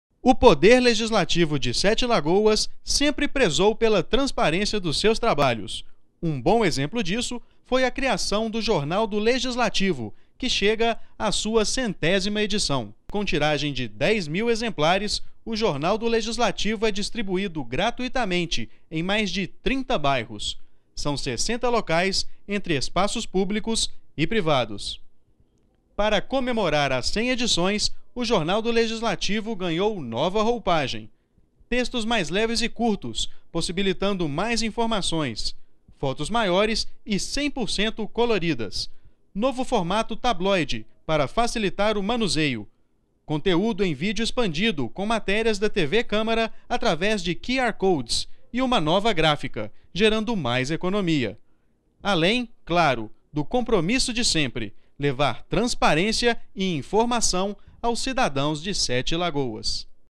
Jornal do Legislativo completa 100 edições
O Poder Legislativo de Sete Lagoas sempre prezou pela transparência dos seus trabalhos. Um bom exemplo disso foi a criação do Jornal do Legislativo, que chega a sua 100ª edição.
Saiba mais: